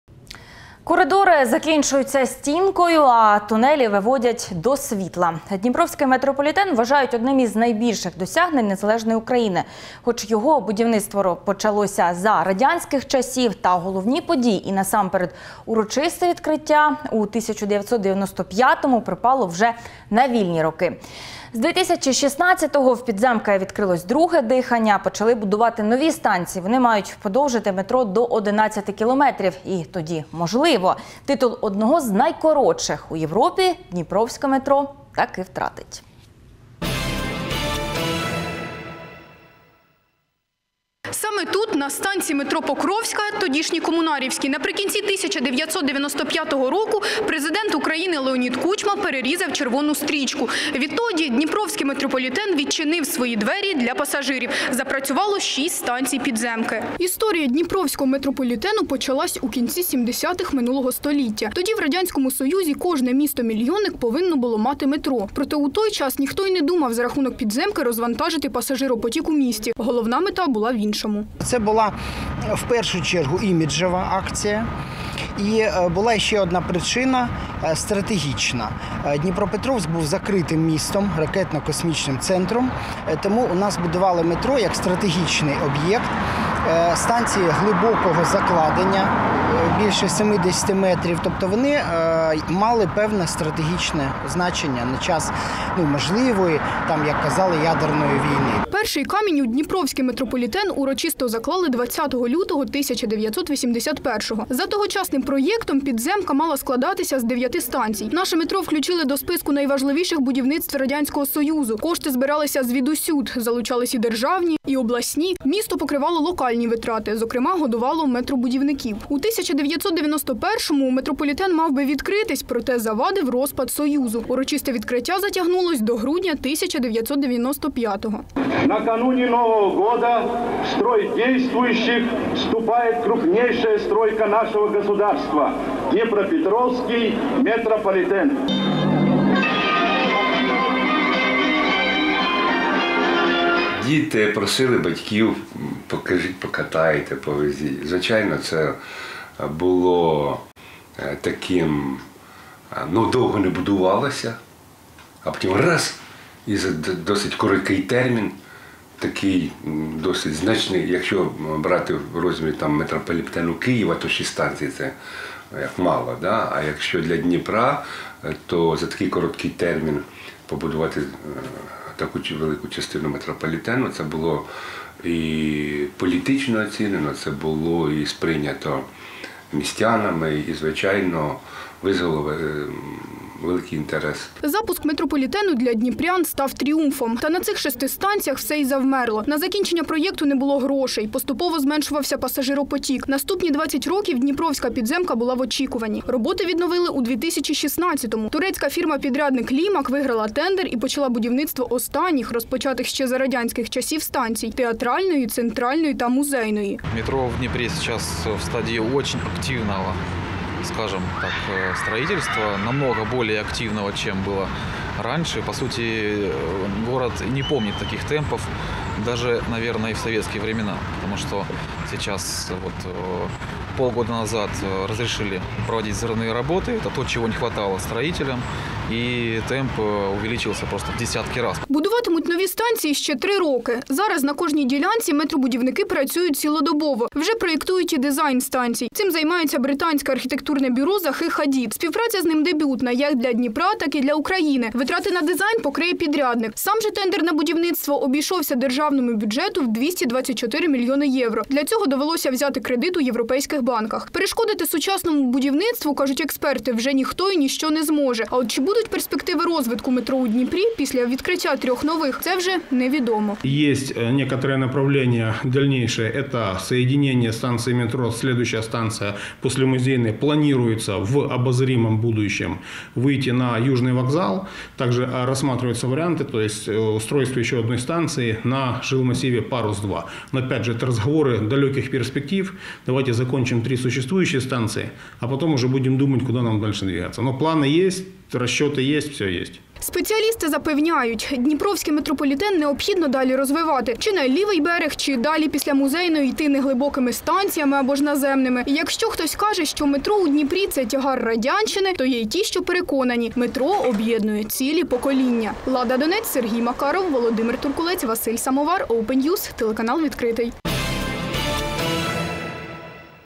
Який шлях пройшов дніпровський метрополітен до незалежності
Коридори закінчуються стінкою, а тунелі виводять до світла. Дніпровський метрополітен вважають одним з найбільших досягнень незалежної України. Хоч його будівництво почалося за радянських часів, та головні події, і насамперед урочисте відкриття у 1995-му, припало вже на вільні роки. З 2016-го в підземки відкрилось друге дихання - почали будувати нові станції. Вони мають подовжити метро  від 7 до 11 кілометрів. І тоді, можливо, титул “одного з найкоротших у Європі” дніпровське метро таки втратить.